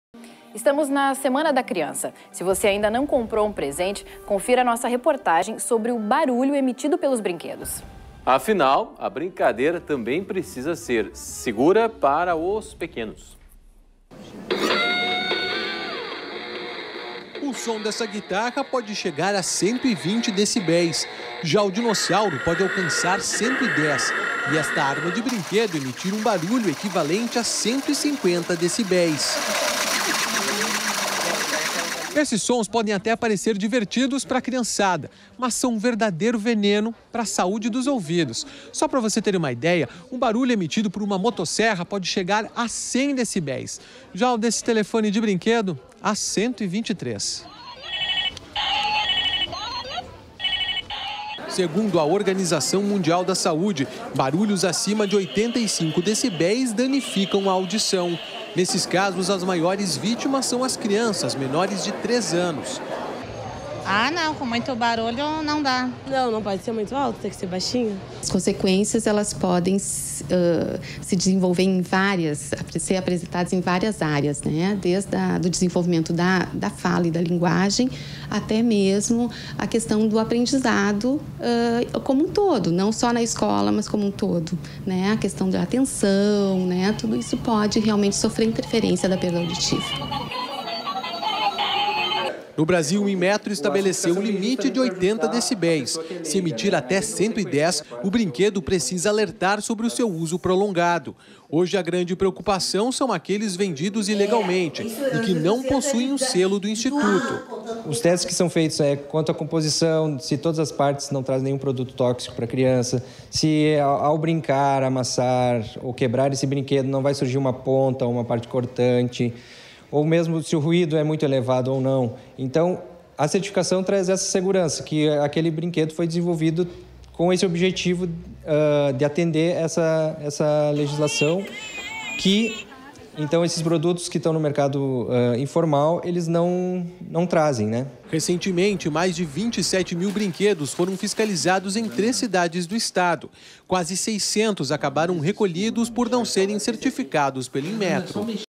Brinquedos barulhentos podem até ser atraentes, mas são nocivos a audição das crianças
Estamos na semana da criança. Se você ainda não comprou um presente, confira nossa reportagem sobre o barulho emitido pelos brinquedos. Afinal, a brincadeira também precisa ser segura para os pequenos.